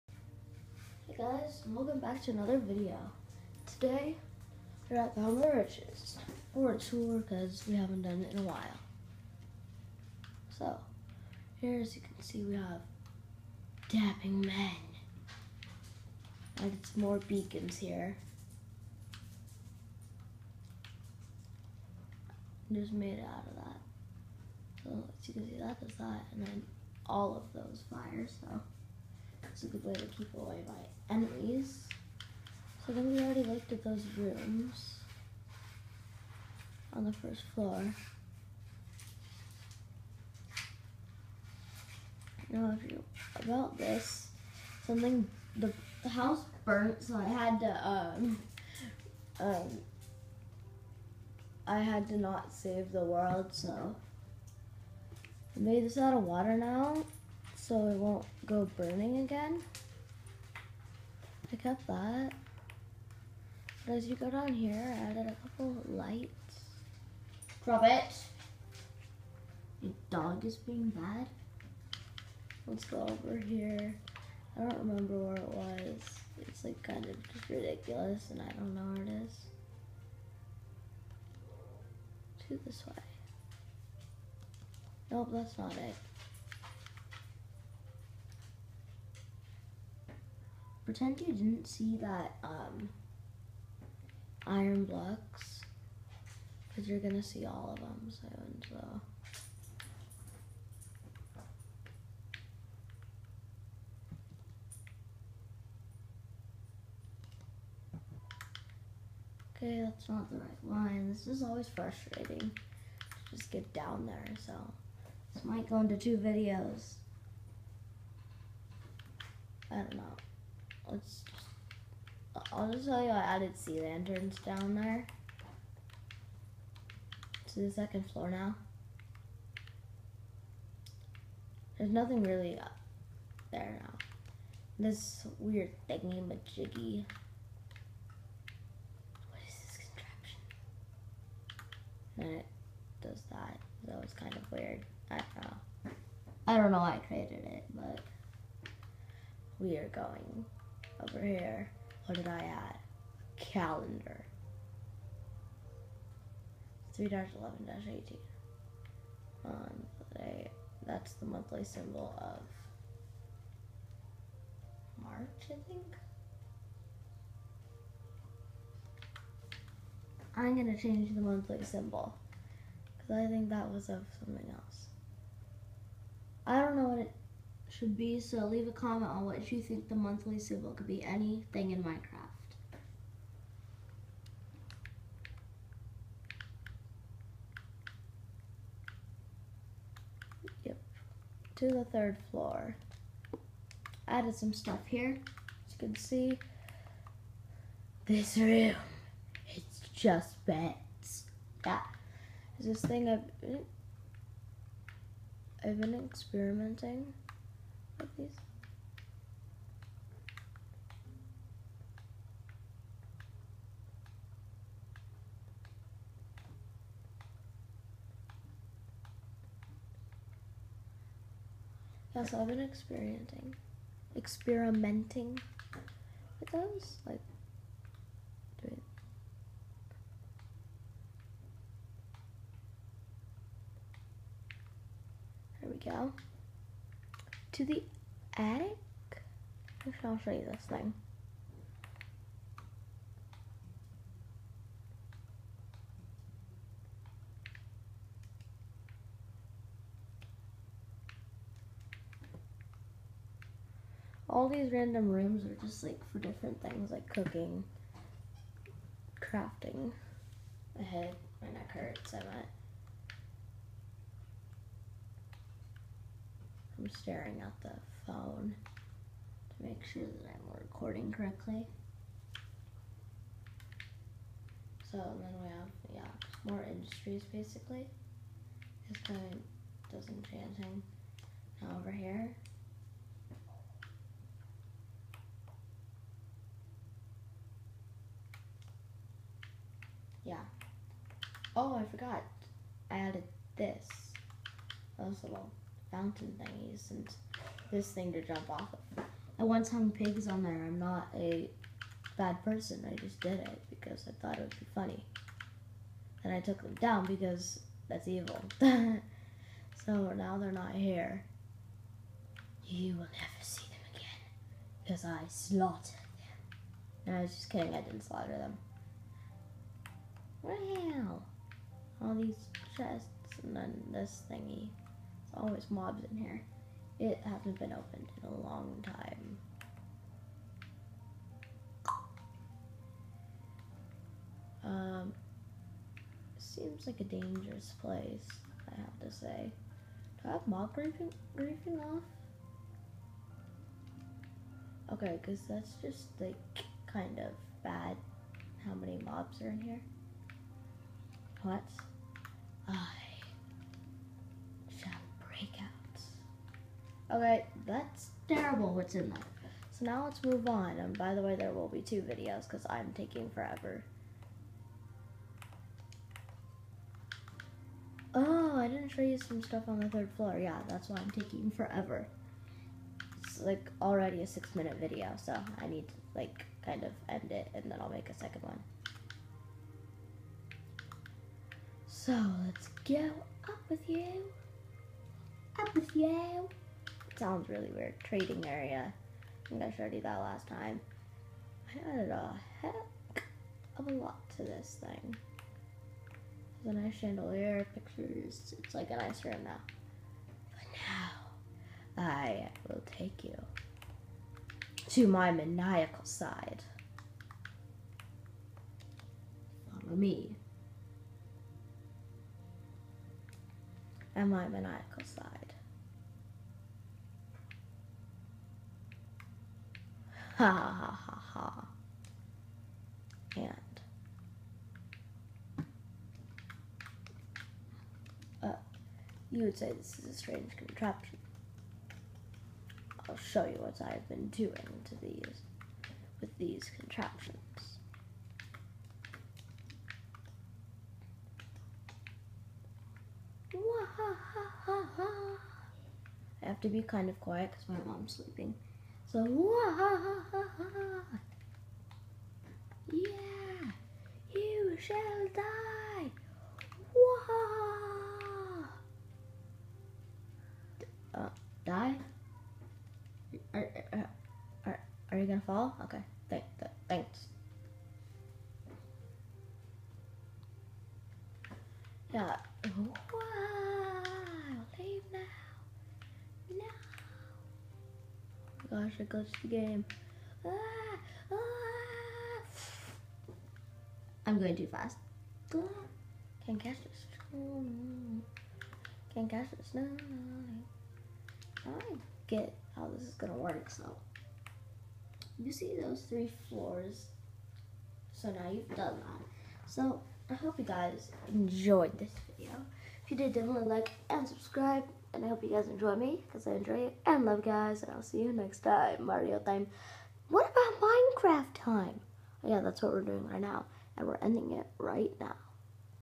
The Home of the Riches update floors 1-3 and attic with prison
Today we are exploring the HOTR and it's new updates. There will be another vid about the other half of the house because I only had time to show 1/2 of the HOTR.